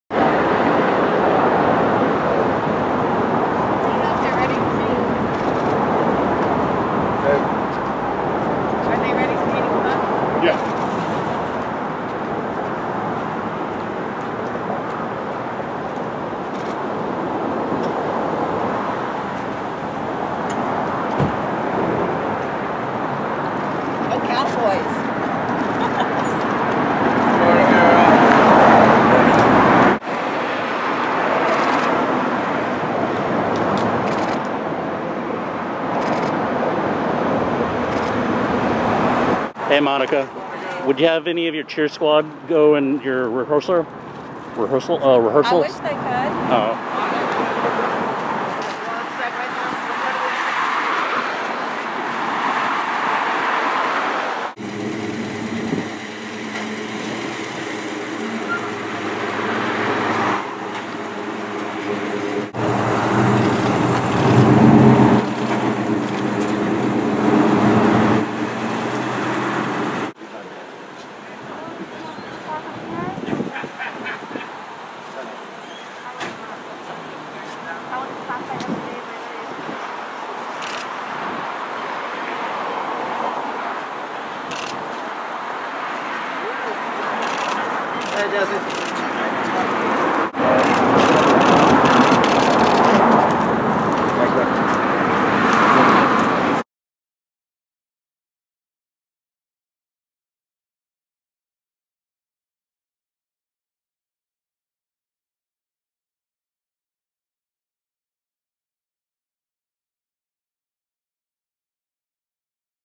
Carole Baskin, Monica Aldama, and Jesse Metcalfe in Great Spirits for DWTS Rehearsals
Carole Baskin, Monica Aldama, and Jesse Metcalfe arriving at the Dancing with the Stars studio in Hollywood for rehearsals on Saturday.